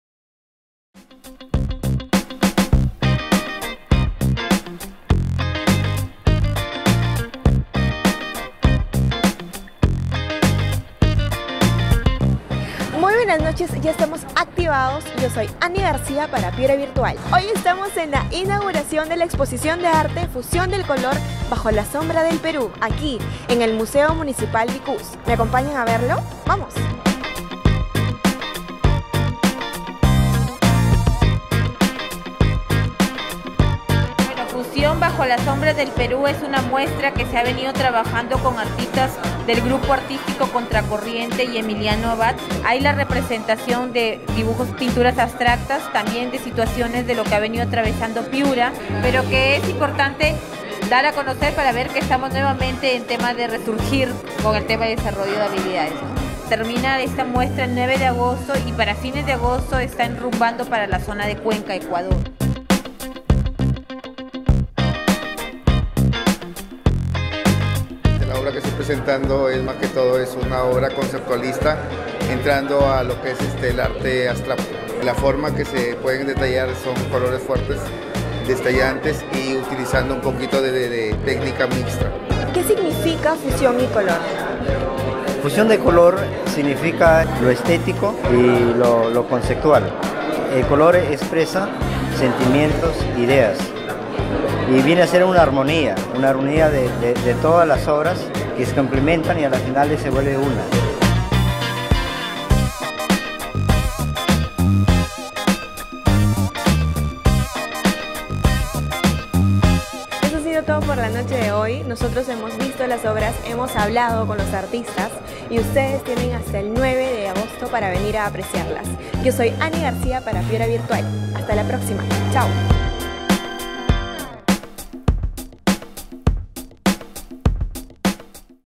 Muestra Plástica "Fusión de color, bajo la sombra del Perú"
Inauguración de la muestra plástica "Fusión de color, bajo la sombra del Perú" en el Museo Vicús de Piura. Esta exposición itinerante estará abierta al público hasta el 9 de agosto.